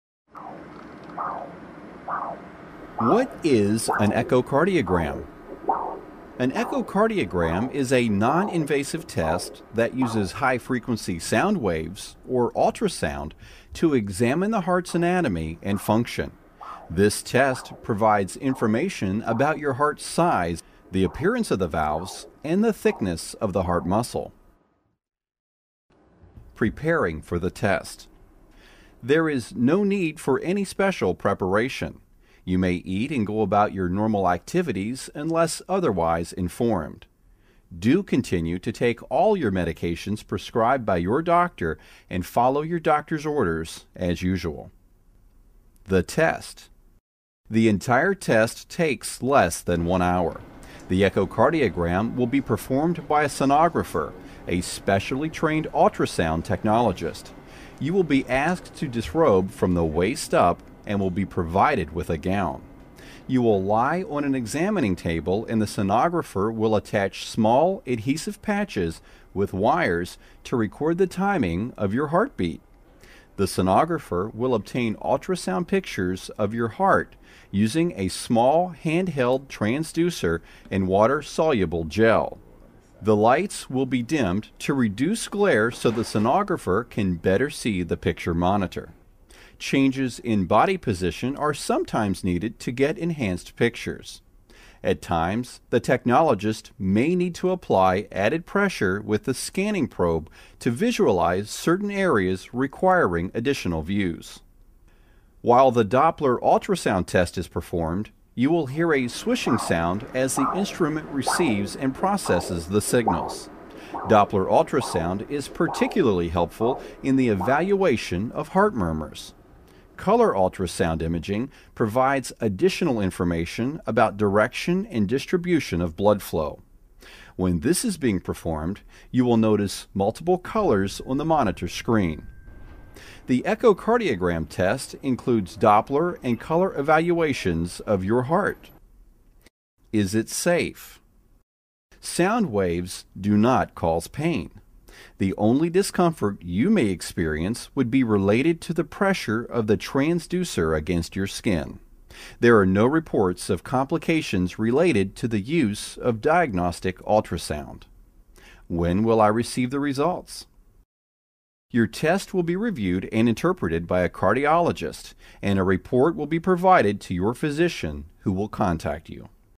echocardiogram.mov
An echocardiogram is a non-invasive test that uses high frequency sound waves, or ultrasound, to examine the heart's anatomy and function.